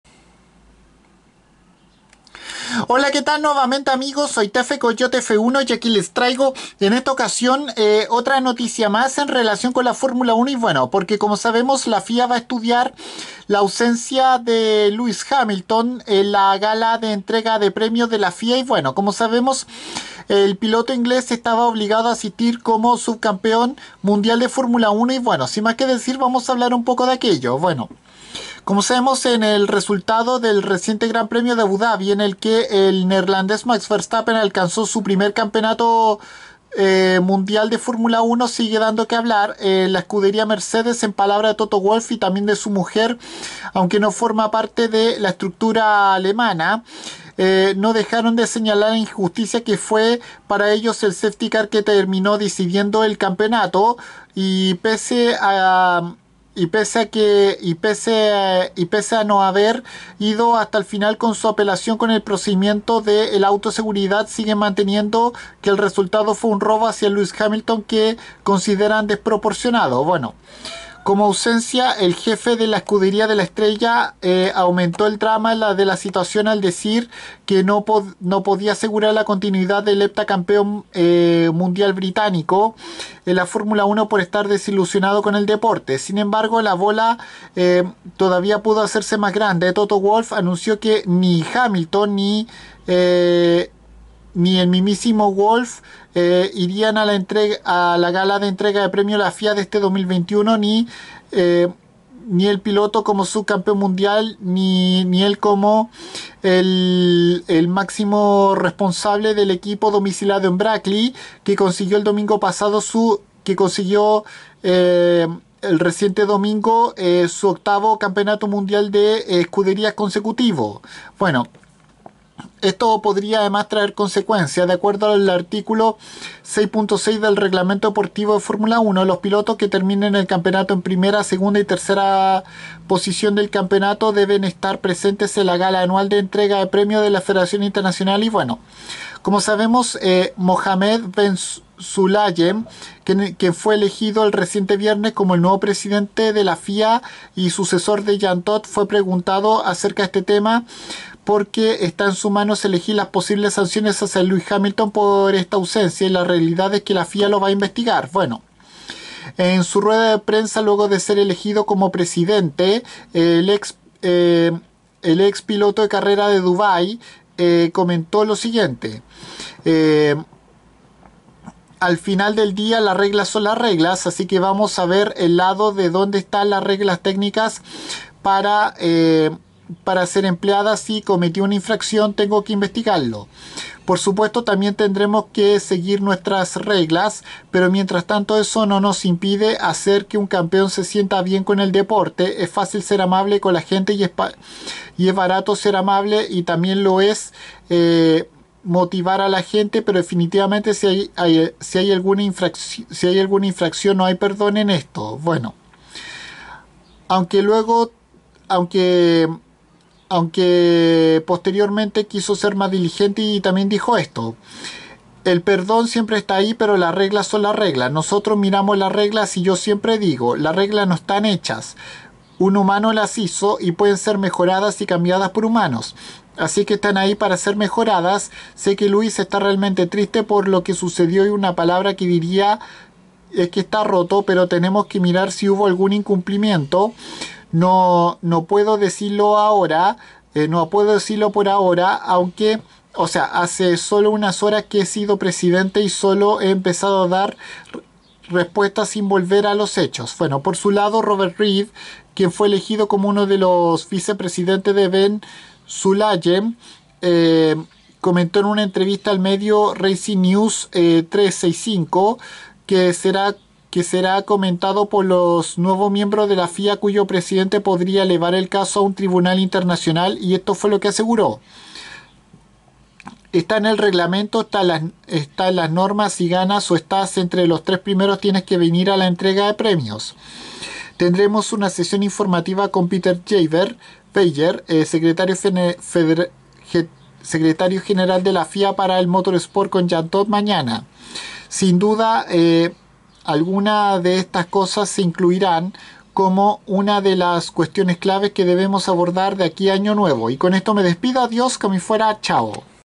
LA FIA estudiará la no presencia de Lewis Hamilton en la gala de entrega de premios
El piloto inglés debió asistir por en la reciente Temporada 2021 de la Fórmula 1 subcampeón mundial